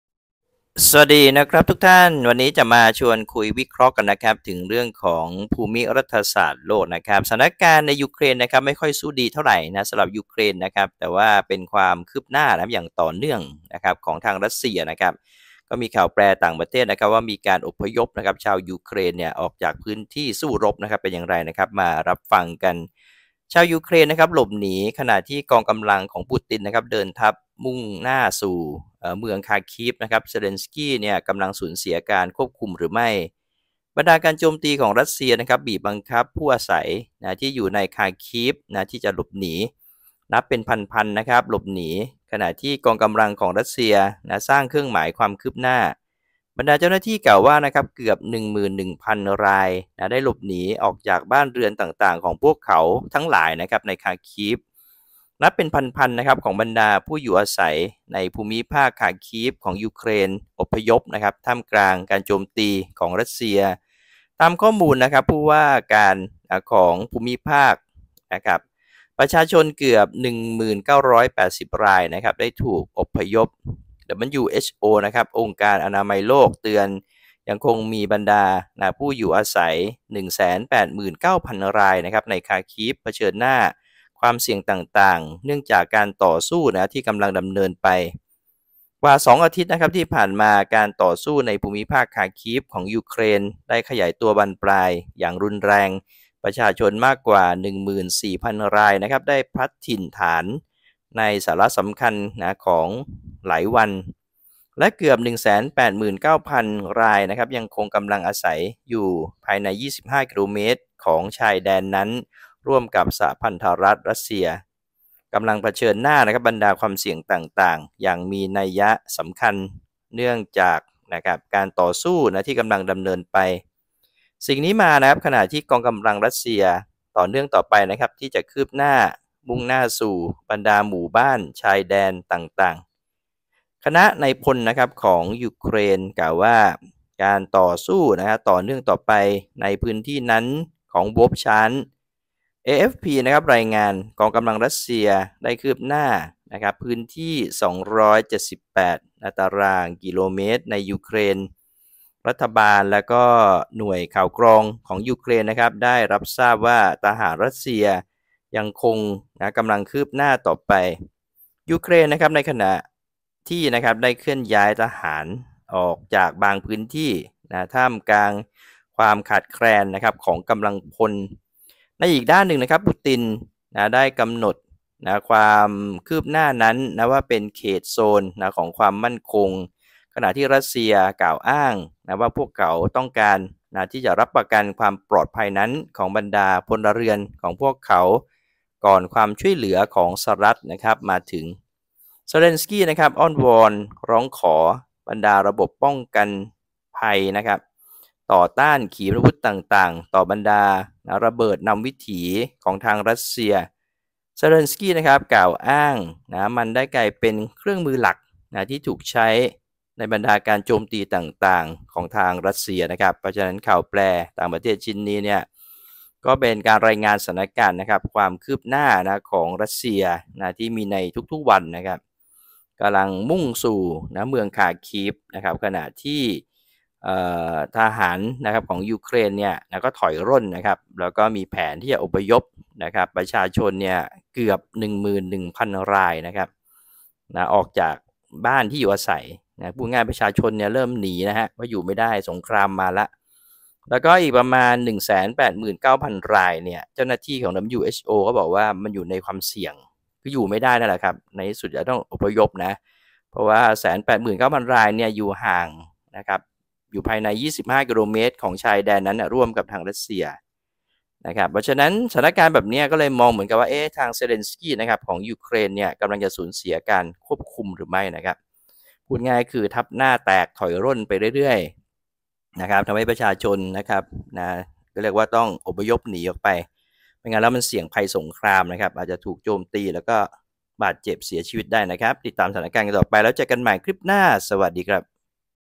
ชาวยูเครนหลบหนีขณะที่กองกำลังของปูตินเดินทัพมุ่งสู่เมืองคาร์คีฟ เซเรนสกี้กำลังสูญเสียการควบคุม?!
วิเคราะห์ เหตุการณ์ สถานการณ์ ภูมิรัฐศาสตร์โลก ชาวยูเครนหลบหนีขณะที่กองกำลังของปูตินเดินทัพมุ่งสู่เมืองคาร์คีฟ เซเรนสกี้กำลังสูญเสียการควบคุม?!